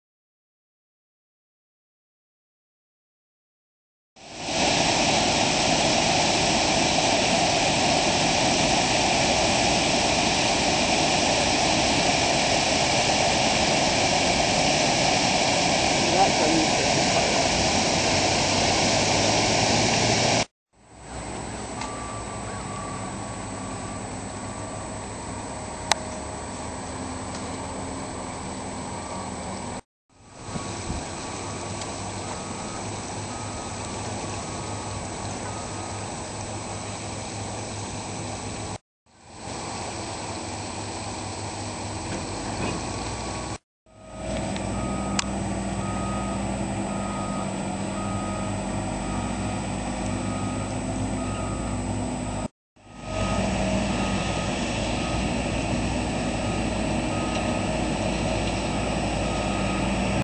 walnut_creek_3-30-10.wmv
Walnut Creek, Erie Pa - water conditions- steelhead fishing
for more steelhead fishing videos and information